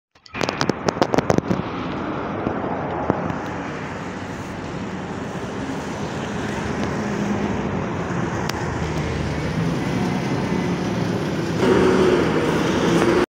Автобус НефАЗ-5299-40-51 № 1973 Маршрут-54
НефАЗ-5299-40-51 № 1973
Госномер: КК 902 66
Группа: ЕМУП Гортранс
Регион: Свердловская область
Локация: Екатеринбург, ЕМУП "Гортранс"
Предприятие: Автобусное предприятие №6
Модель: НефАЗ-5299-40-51
5299UP
Шасси: КАМАЗ-52974
С...: 01.2017
Построен: 2016
Заводской №: 157
VIN: X1F529900G0000157
№ шасси: 529745G4012711
Текущее состояние:  Эксплуатируется
Назначение: Линейный

⚠️Всем привет и удачного просмотра!